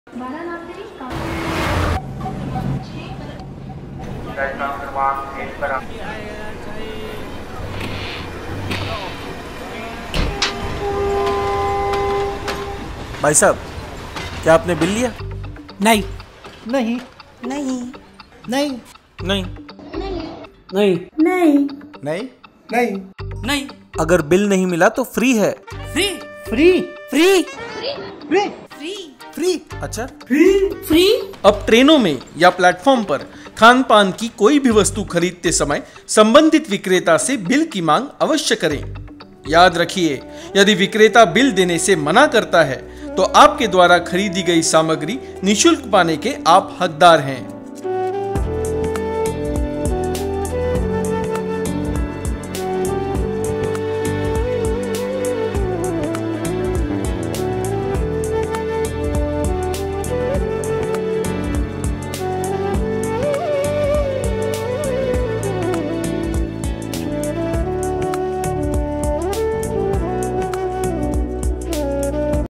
Bill nahi to free | No bill no payment | Campaign by Central Railway
Video Courtesy: Central Railway with intention to promote this Honest cause for nation.

So next time if vendor in railway platform or inside the train doesnt give you bill, Take it for free.
Right move for safety of people and national economy.